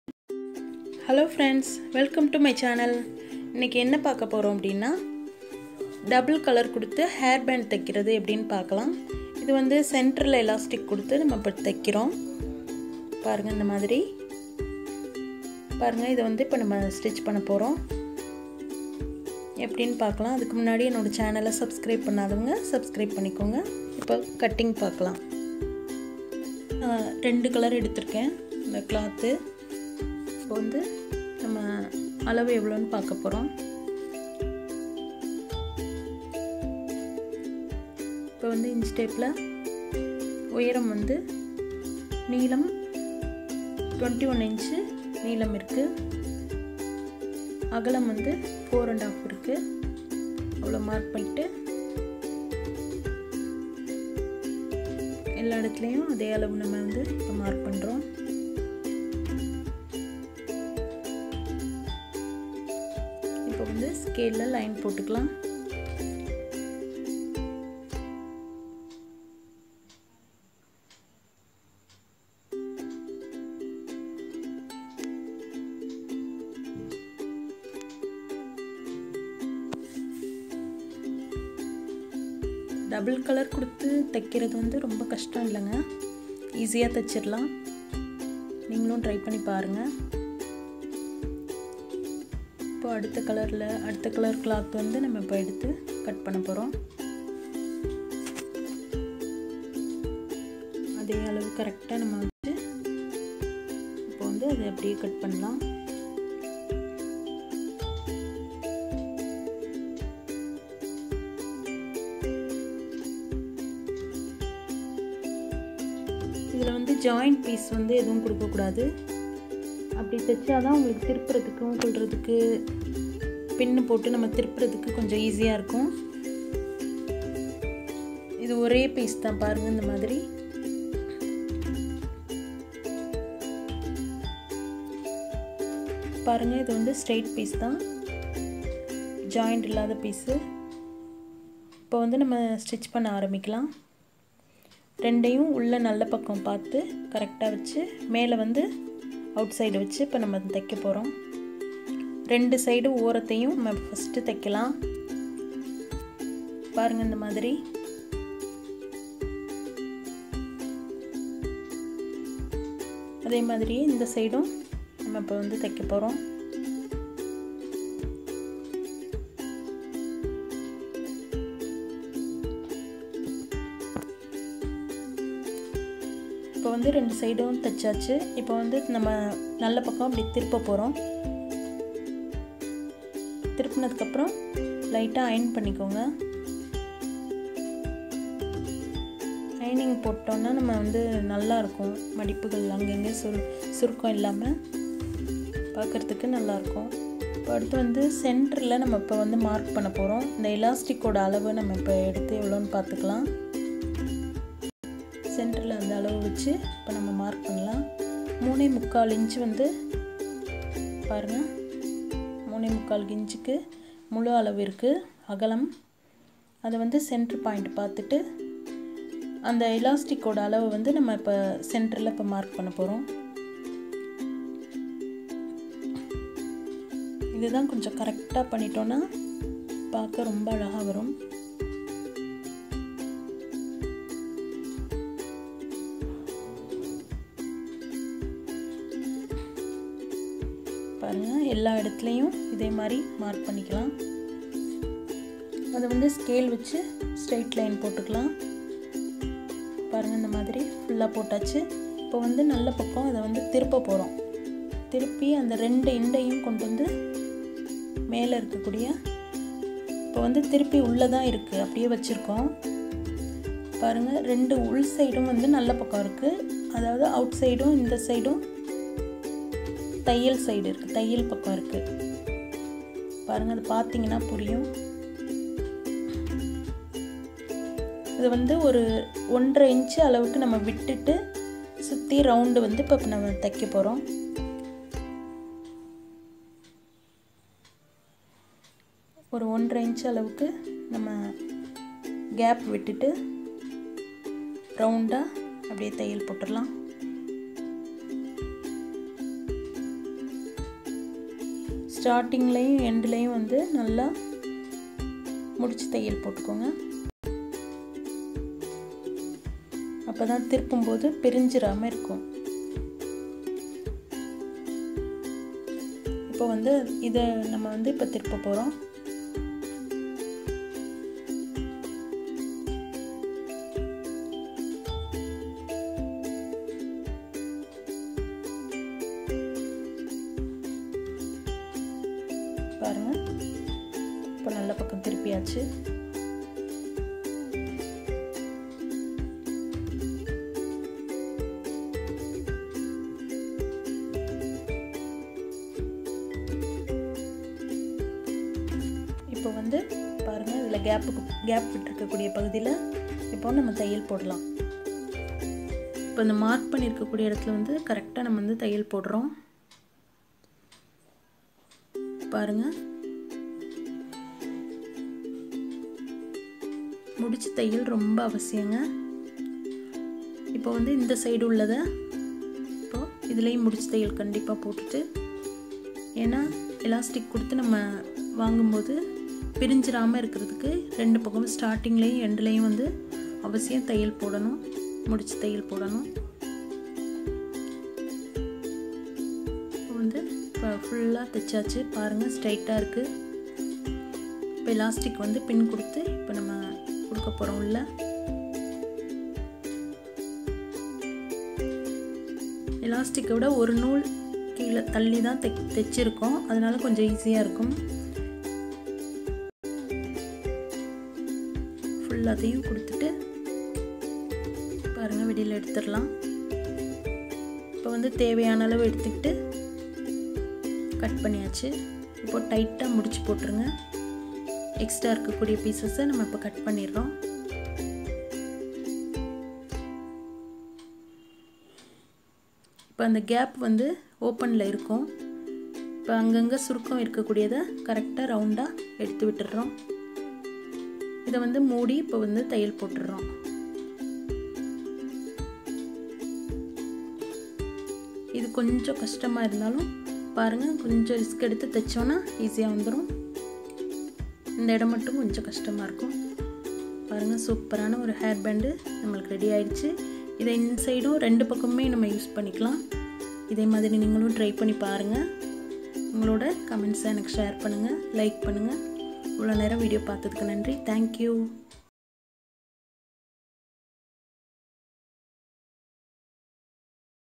How to stitch hair band/easy made hair band/diy hair band stitching
Welcome everyone to Shafin tailoring class...
In this video I have showed that how to stitch a hair band easily with a Sewing machine.